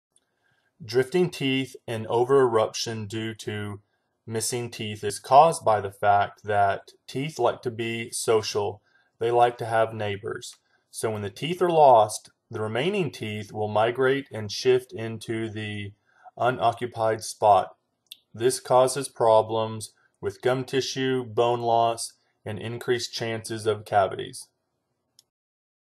Drifting Teeth and Over Eruption of Teeth
Drifting Teeth & Over-Eruption:
The remaining teeth no longer have the support that the loss teeth and bone provided.  With no support the teeth begin to drift and erupt toward the open areas.

Dr. Michael Nugent
3421 Burke Rd, Pasadena, Texas 77504